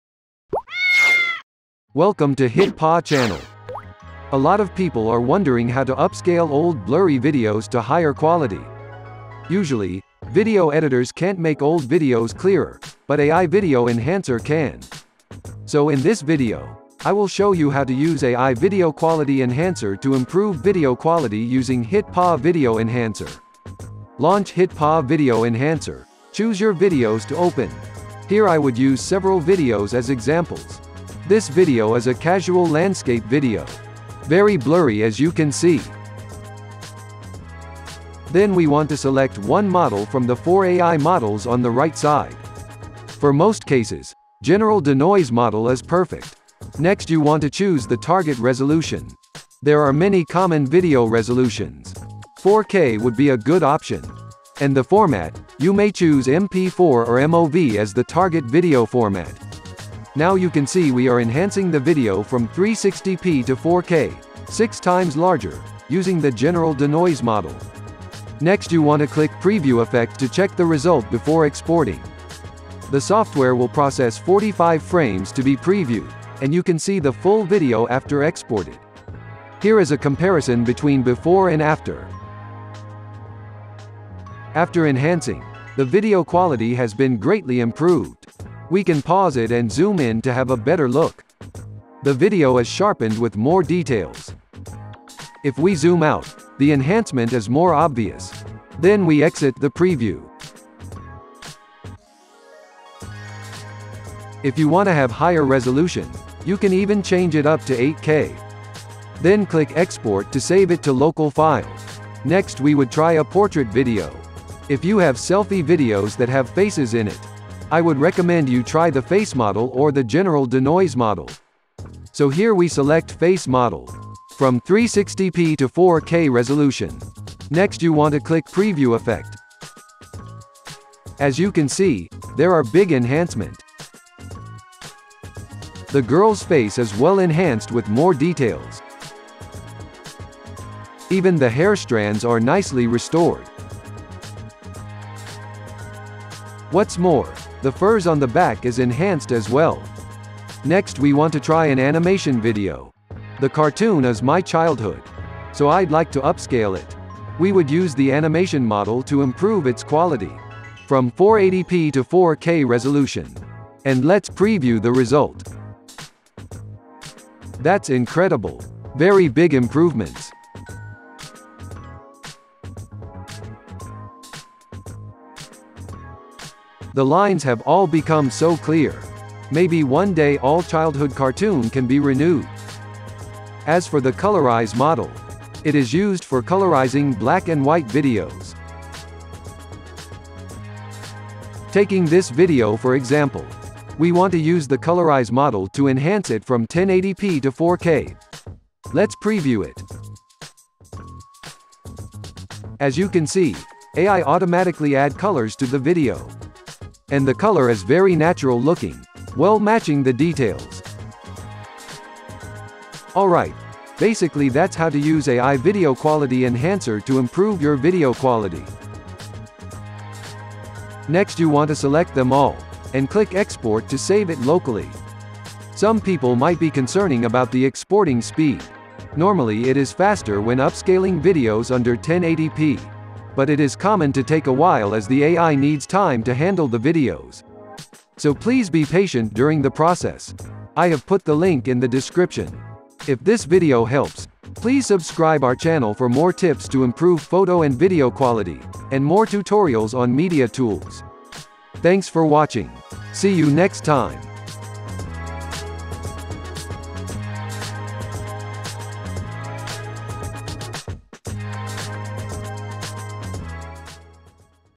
(2023) How to Use Video Quality Enhancer Software to Improve Video Quality | HitPaw Video Enhancer
In this video, I will show you how to use AI video quality enhancer to improve video quality using HitPaw Video Enhancer.